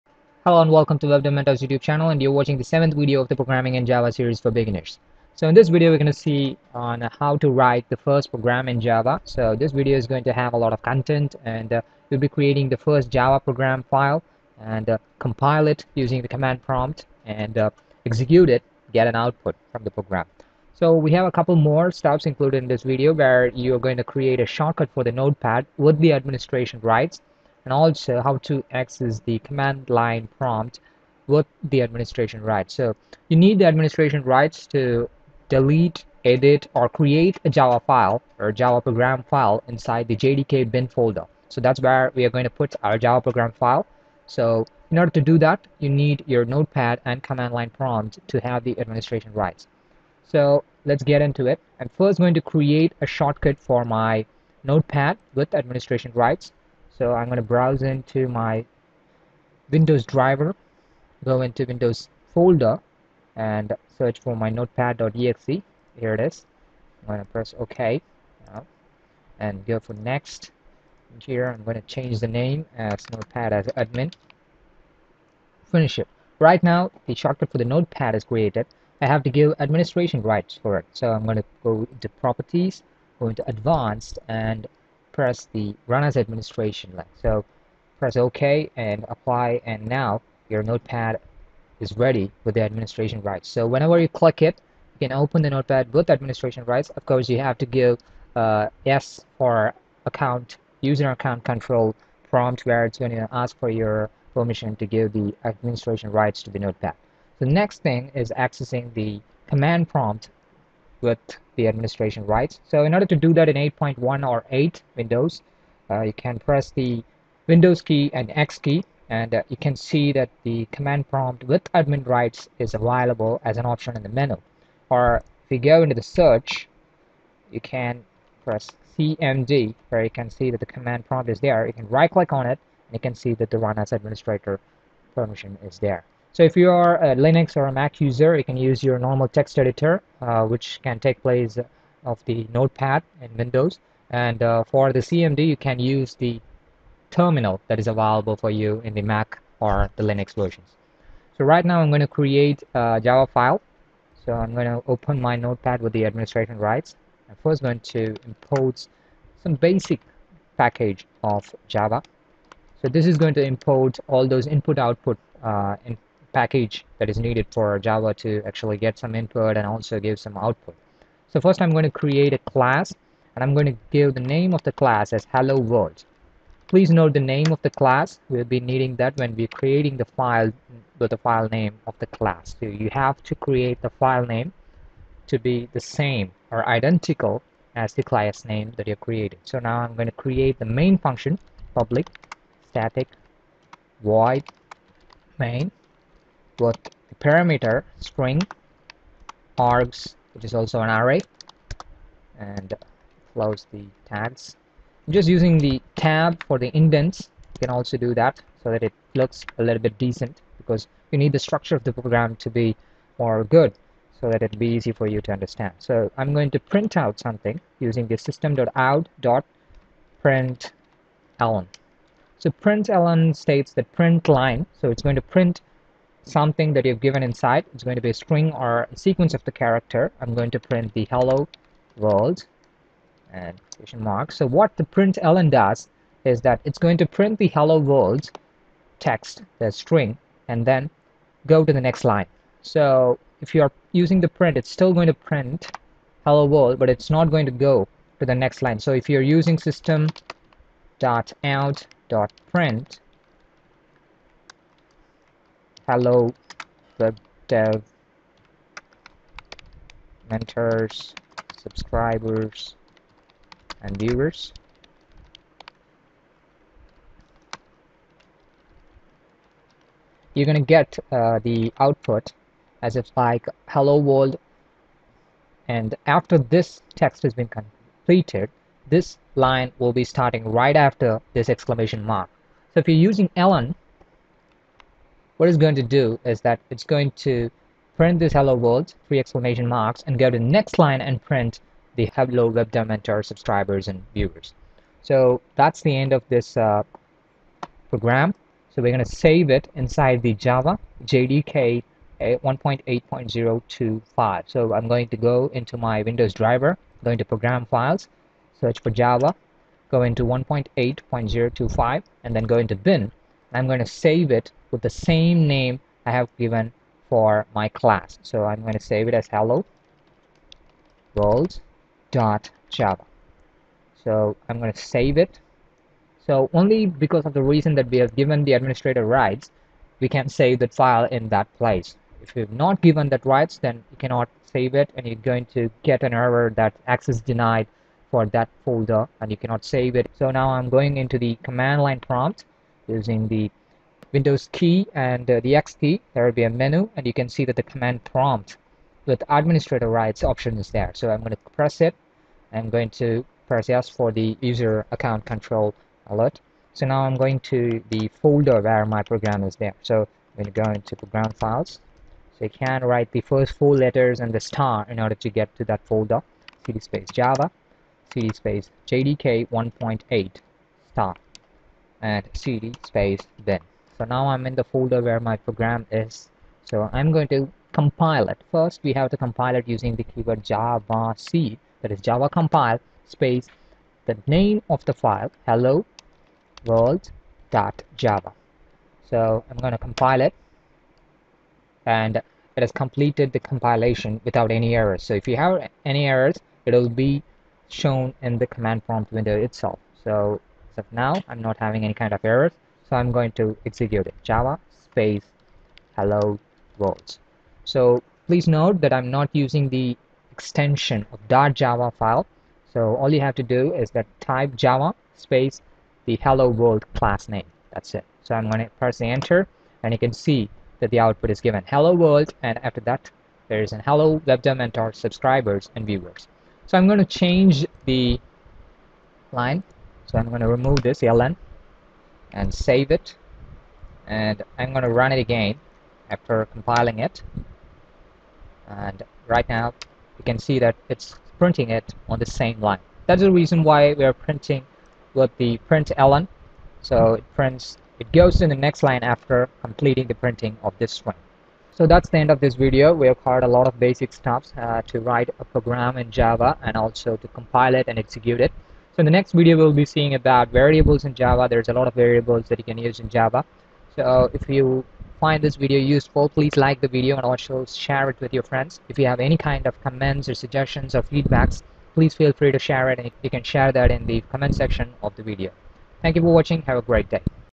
7. Writing your first Program in Java
Learn to write your first program in Java. In this video we are going to learn to write the first program in java with basic tools and applications like notepad or any text editors(comes with OS) and Java development Kit JDK.

The video shows you how to write, compile and execute your first java program.

by
Vasandkumar